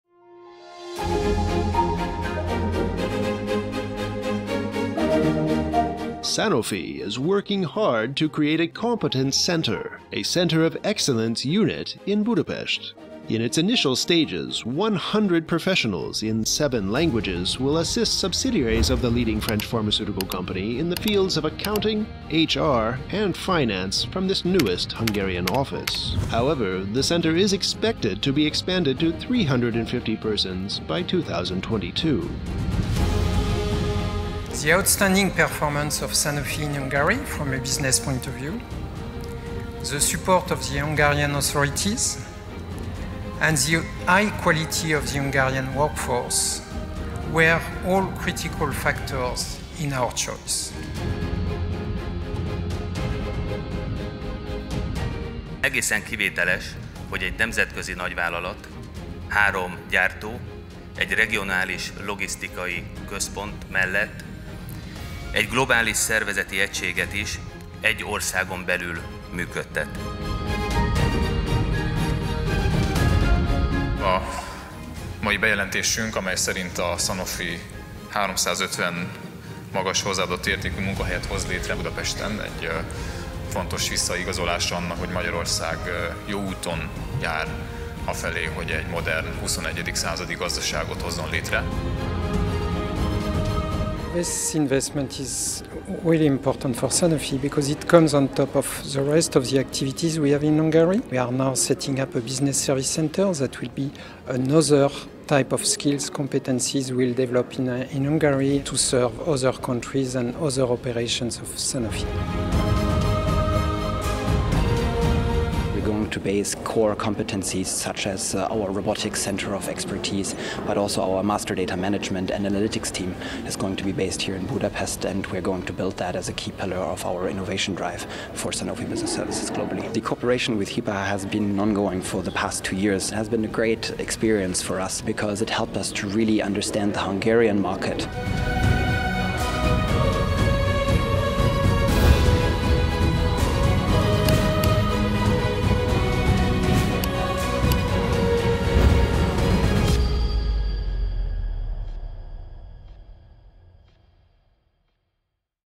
HIPA NEWS - Sanofi opens its European competence centre in Budapest
Sanofi is to create a competence centre, a Centre of Excellence in Budapest. Initially, 100 high added value professionals will assist the subsidiaries of Sanofi in the field of accounting, HR and finance in seven languages in the latest Hungarian unit of the leading French pharmaceutical company, however, the centre will be expanded to 350 persons by 2022.